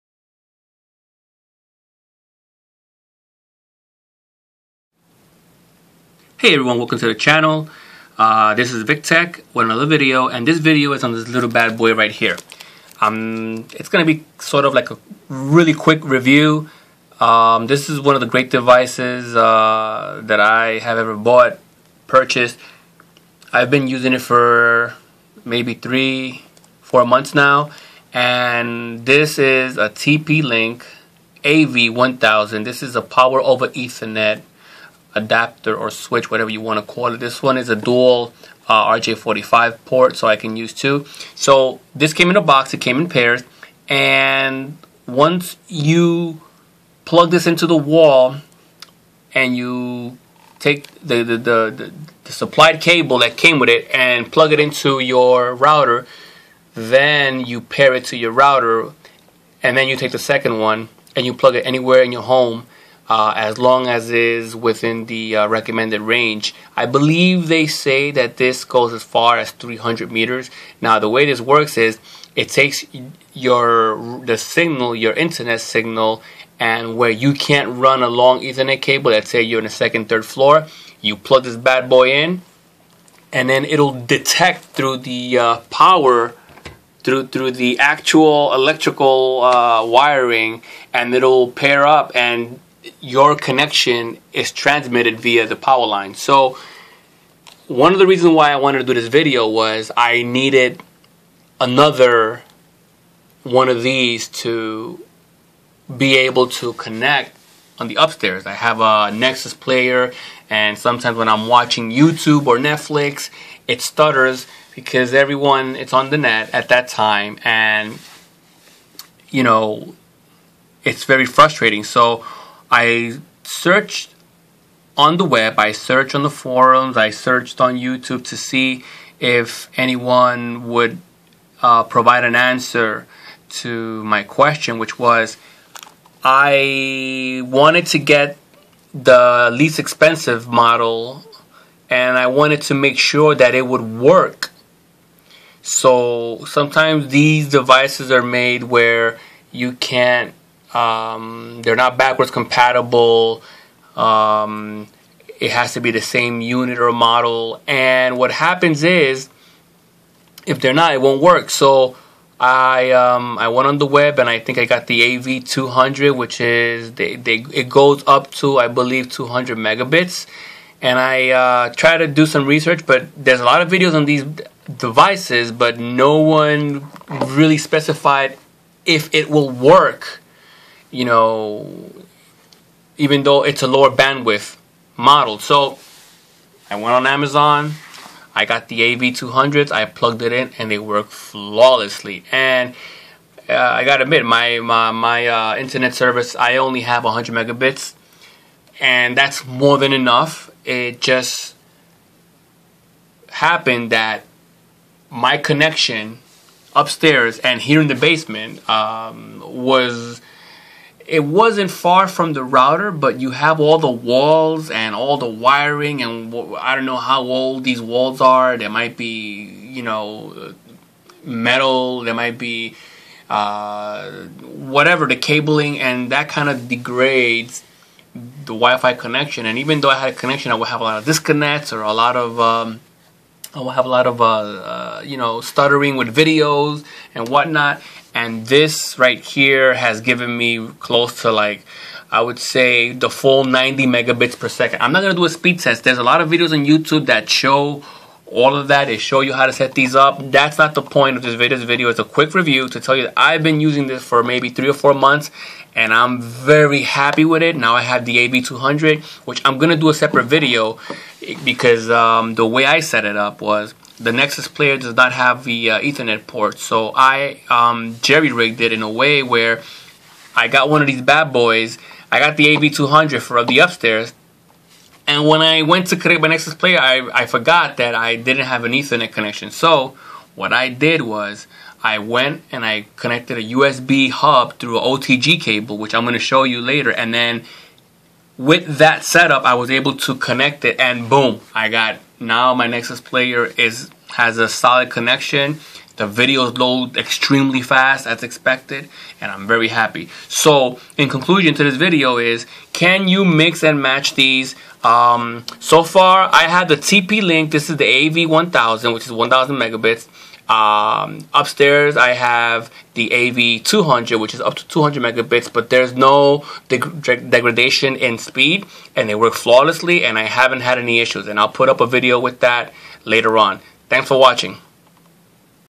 TP-Link AV1000 Can you mix and match?
Can you mix and match different speed POE / Powerline adapters?